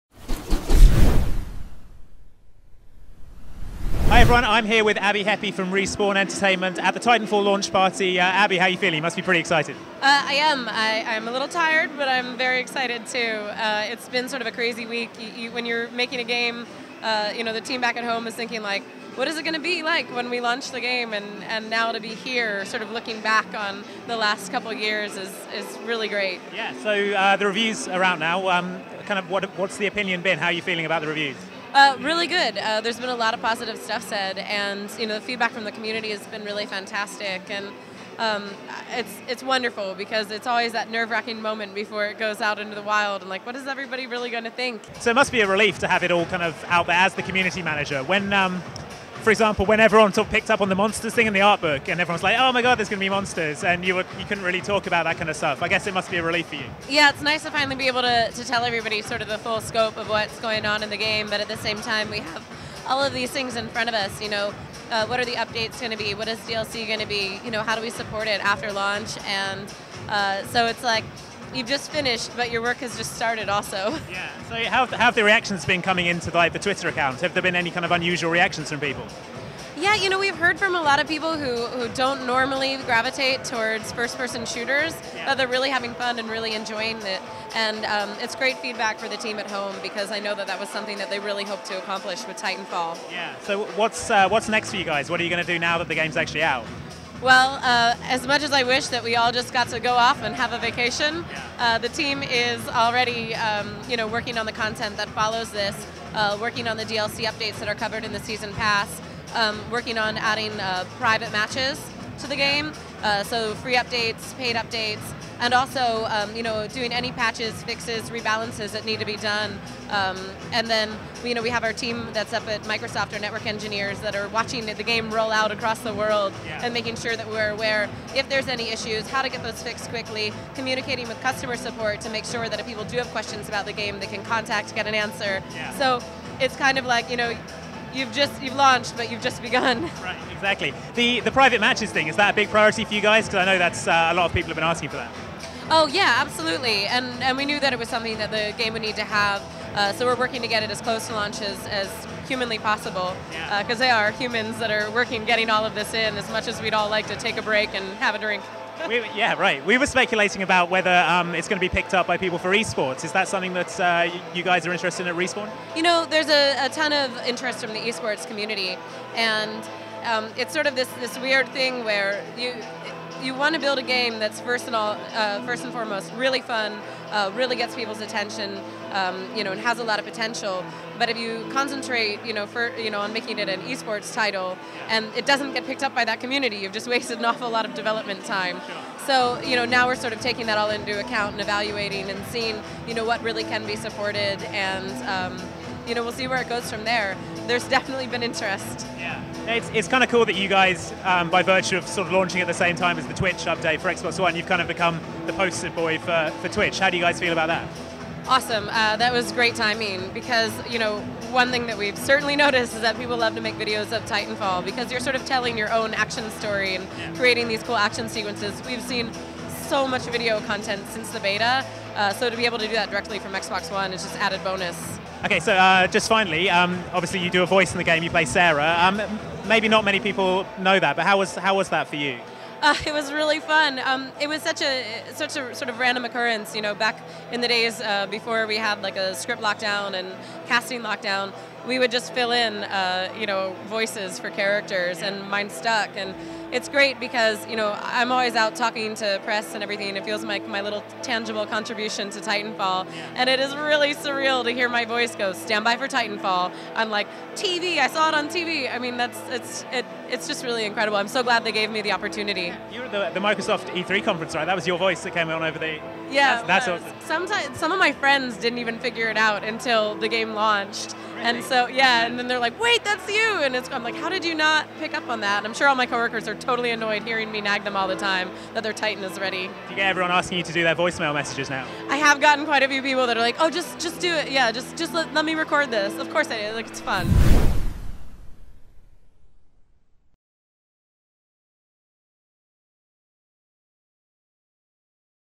What Next For Titanfall? Private Matches And More
Now Titanfall is in people's hands, we spoke to Respawn's Abbie Heppe to find out what we can expect post-release. Private matches, DLC and the season pass are all discussed, plus we chat about Abbie's voice acting debut as Sarah in Titanfall.

Outside Xbox brings you daily Xbox 360 and Xbox One videos. Join us for gameplay, previews, tips, guides, interviews and more.